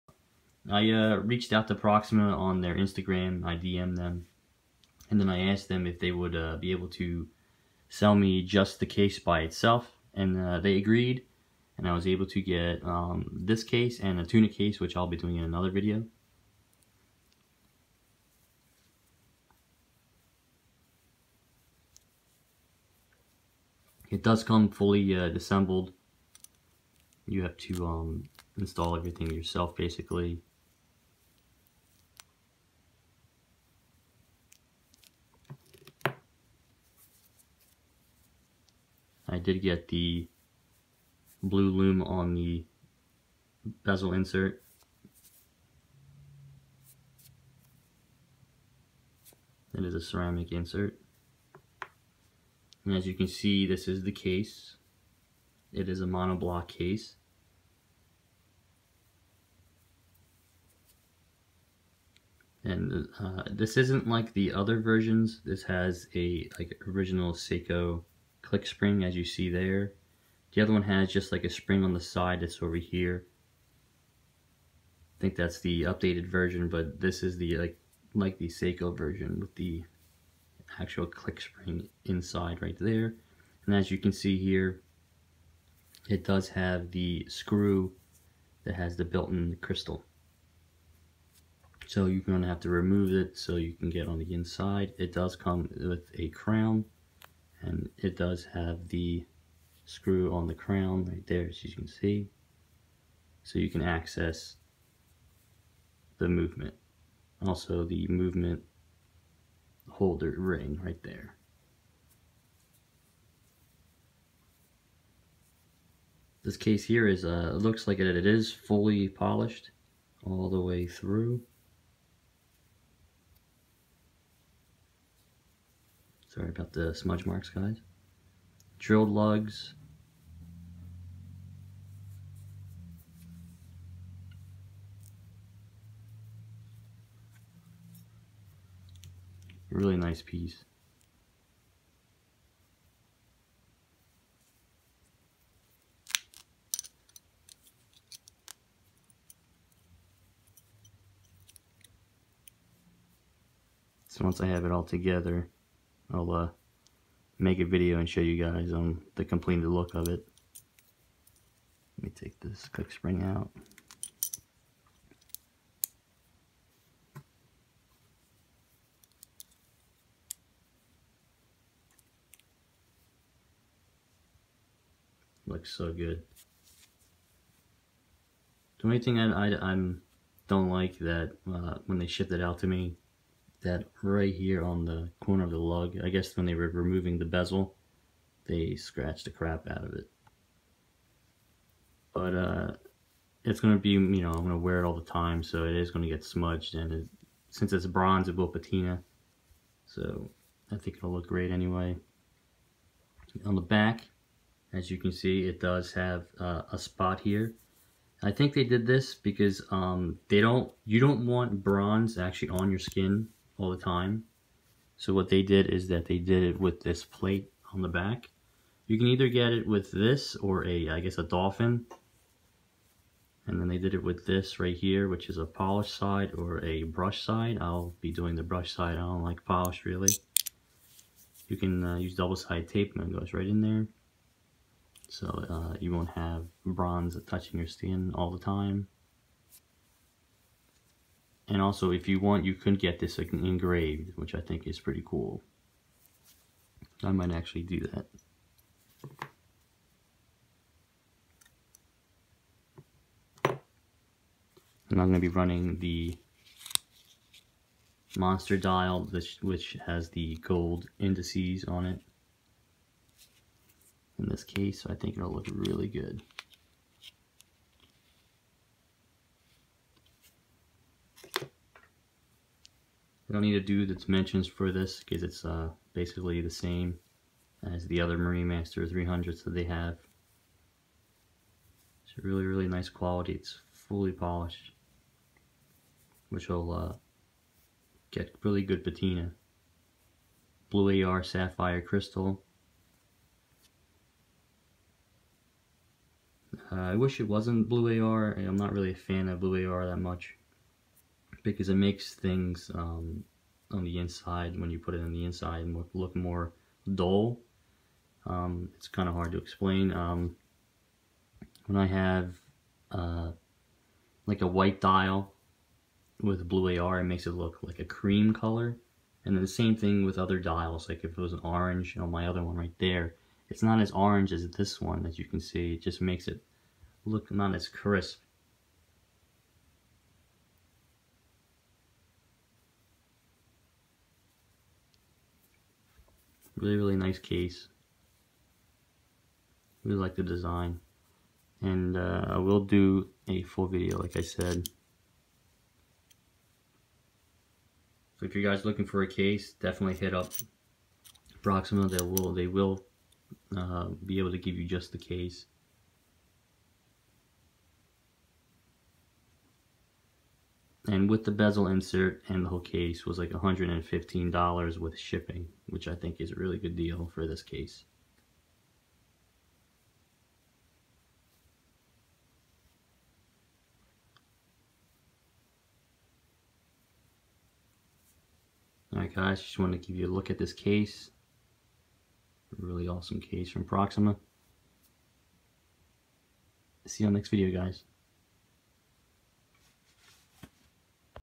Proxima MM300 Bronze Case
A quick look at the Bronze MM300 case from proxima. I messaged them and was able to purchase just the case. Proxima is doing a really wonderful job with these cases.